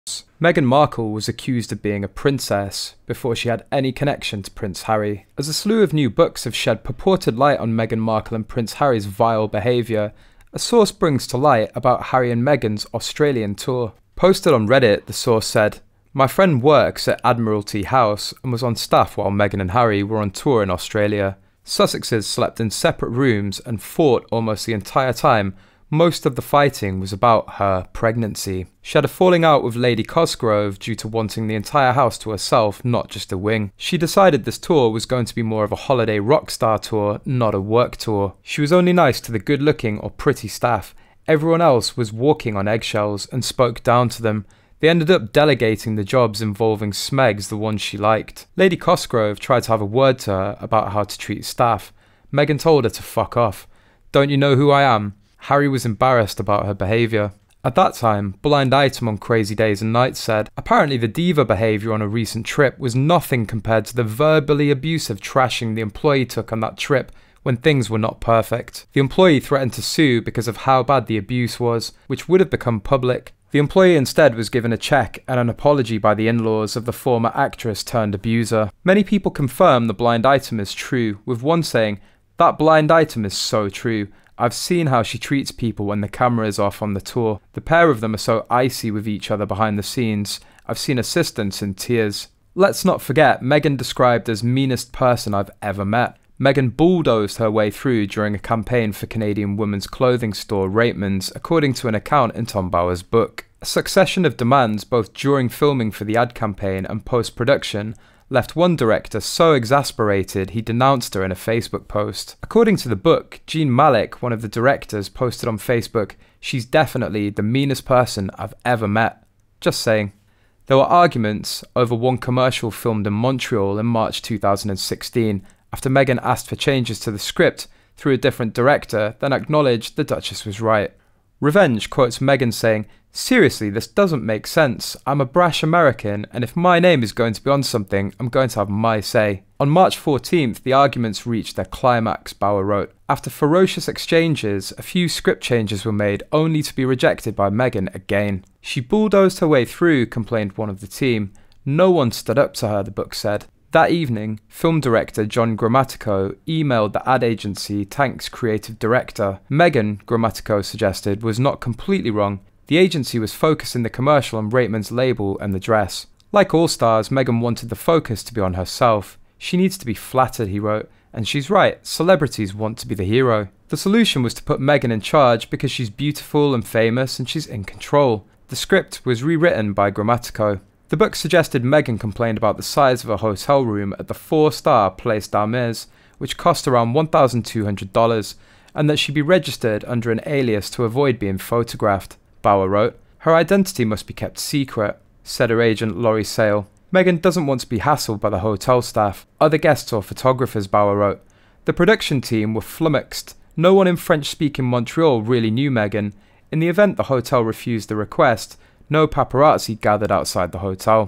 Truth About Australia Tour Comes Out! Harry, Meghan FOUGHT The Entire Time, Slept In SEPARATE Rooms
A succession of demands both during filming for the ad campaign and post-production left one director so exasperated he denounced her in a Facebook post.
According to the book, Jean Malek, one of the directors, posted on Facebook "She is definitely the meanest person I've ever met. Just saying."

There were arguments over one commercial, filmed in Montreal, in March, 2016, after Meghan asked for changes to the script though a different director then acknowledged the duchess was right.